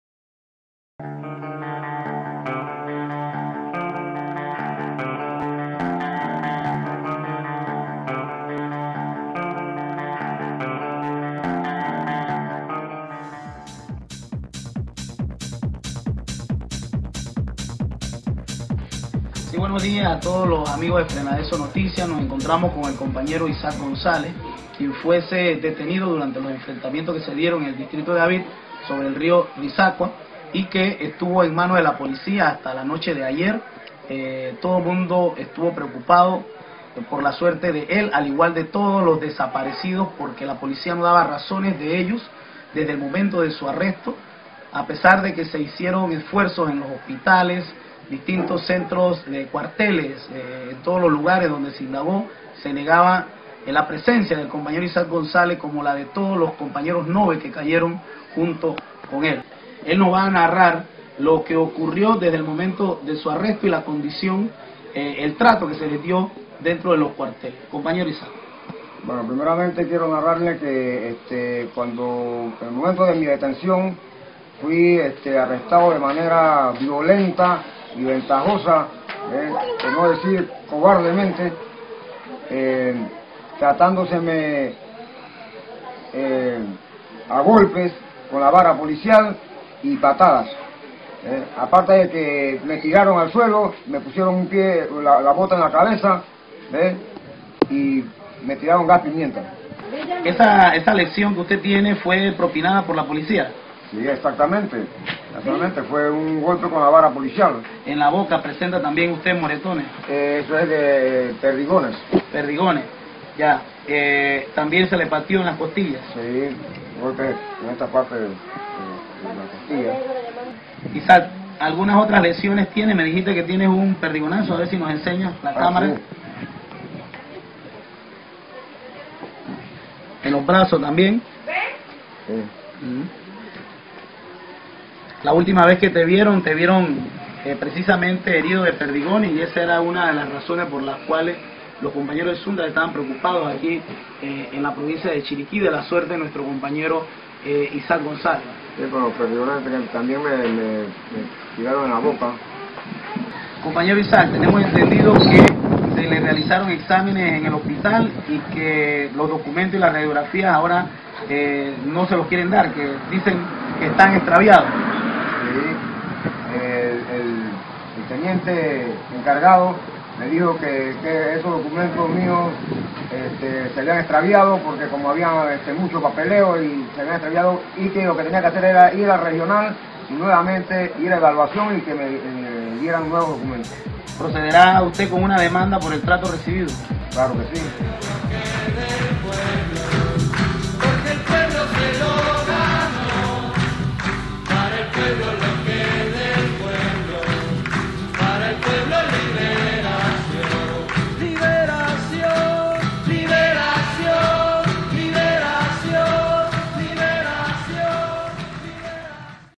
Testimonio de la Brutal represión.mp4
Testimonio de la Brutal represión de la policía Nacional en Panamá
Isaac González / Obrero
Víctima de la Brutal Represión